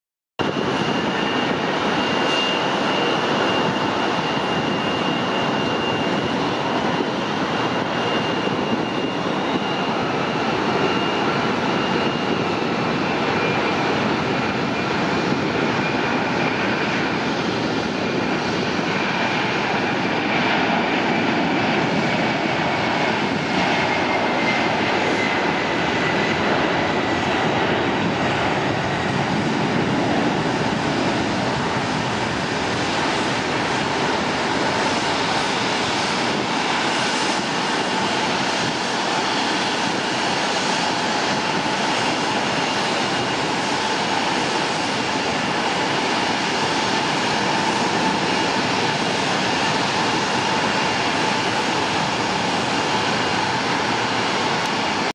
Amazon Air (ATI) Boeing 767-319(ER)(BDSF) [N491AZ] Takeoff from PDX
Airline: Amazon Air (ATI)
Aircraft: 1994 Boeing 767-319(ER)(BDSF)
Reg: N491AZ
Callsign: 8C3313 | ATN3313
Route: Portland (PDX) - Anchorage (ANC)
Livery: Standard